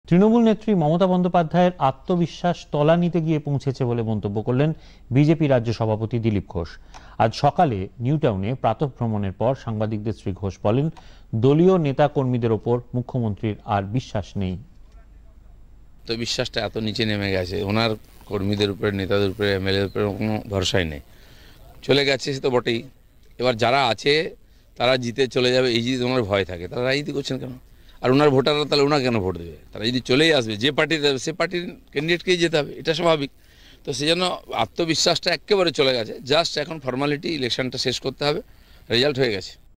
দিলীপ ঘোষের কটাক্ষ
মুখ্যমন্ত্রী মমতা বন্দ্যোপাধ্যায়ের আত্মবিশ্বাস তলানিতে গিয়ে পৌঁছেছে, কটাক্ষ বিজেপির রাজ্য সভাপতি তথা সাংসদ দিলীপ ঘোষের।